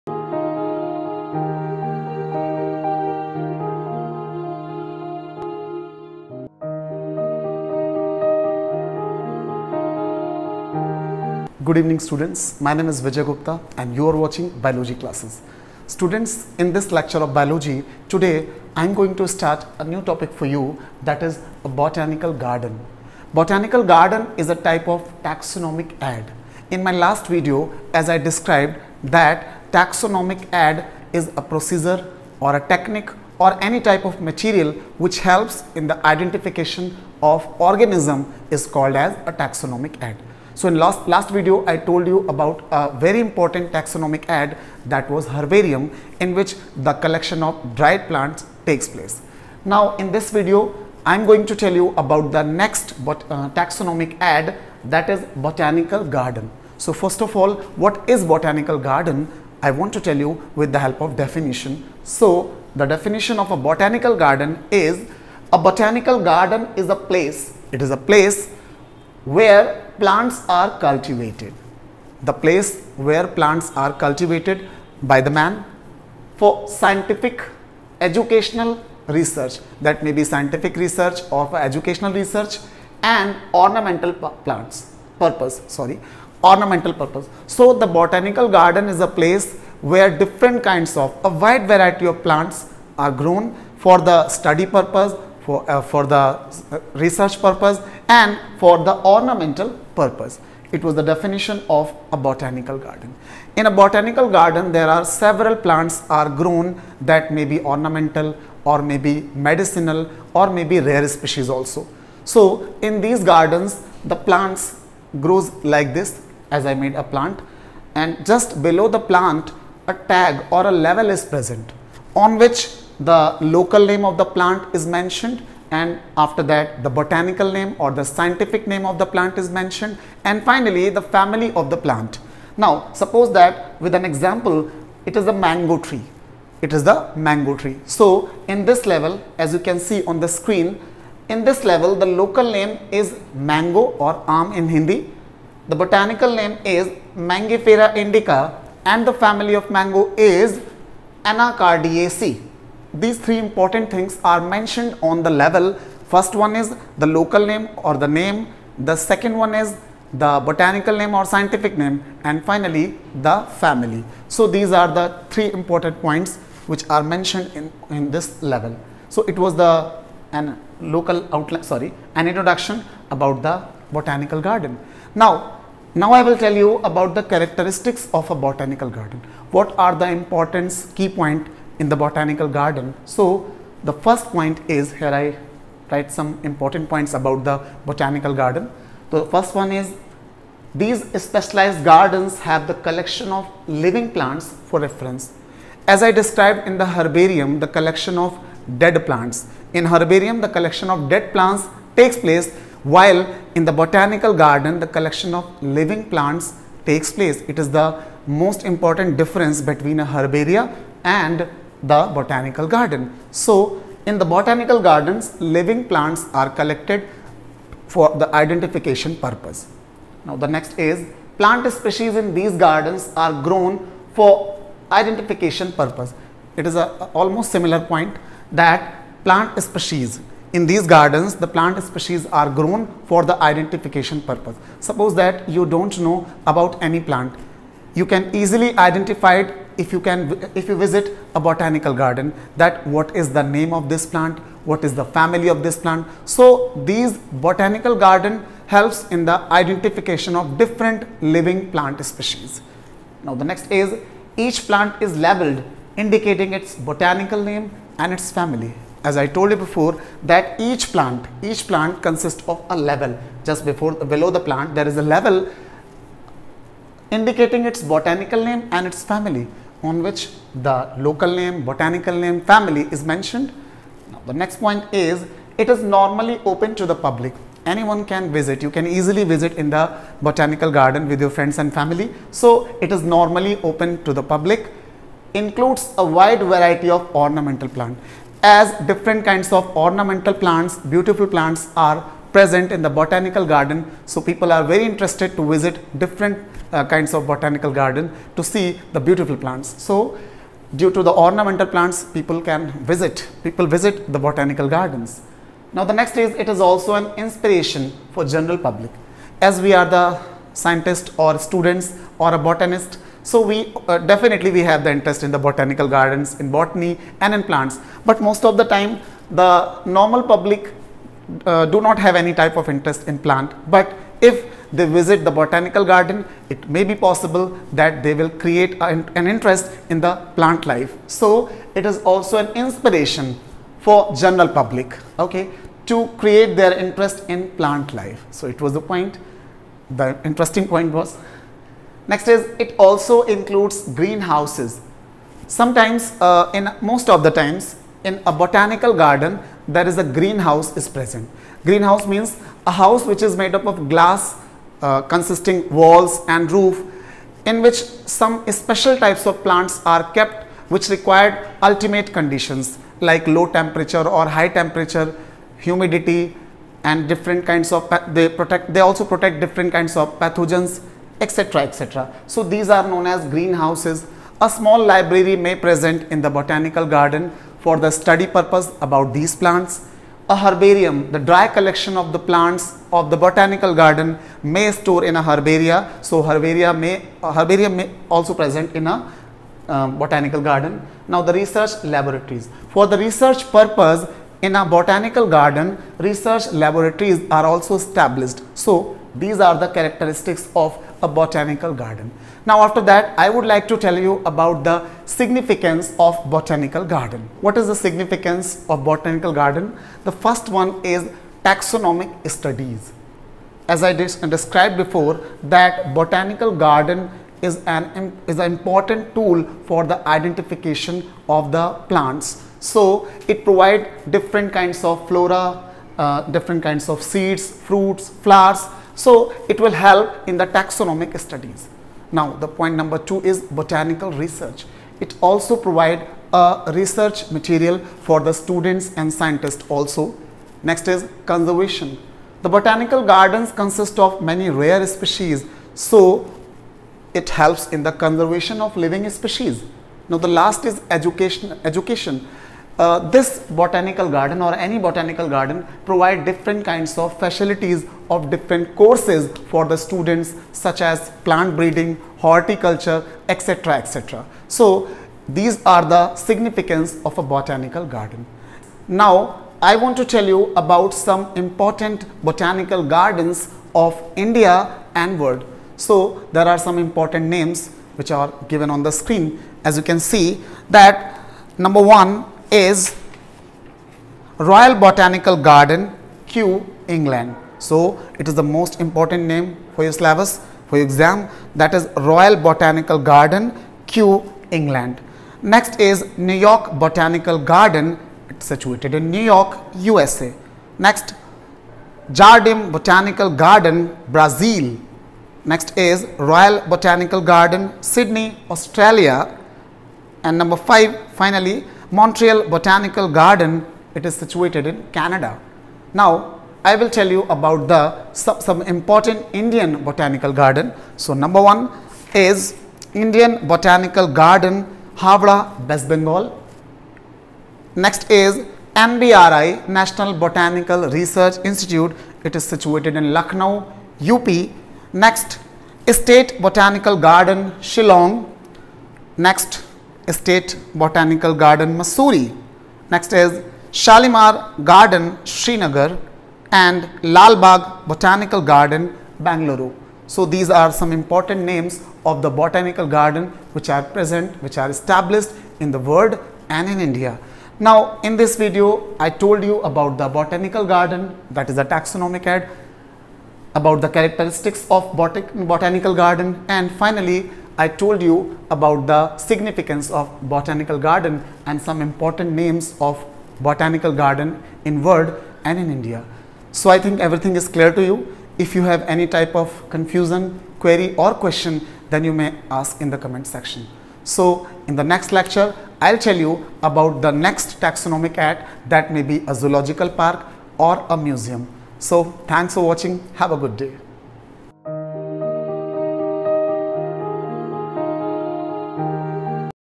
Taxonomic Aids:-Botanical Gardens in Englihs. Class-11 & B.Sc-2 chapter-1. The living world
In this tutorial video the 5th lecture of chapter number-1  (class-11th).( NCERT based)"The living world" is described.

In this lecture Taxonomic aids are described in very easy language.
Botanical gardens in details.

some important Botanical gardens  of India and world are also maintained in this video.
The topic is also important for B.sc -2 students as well as for all the medical competitive examination.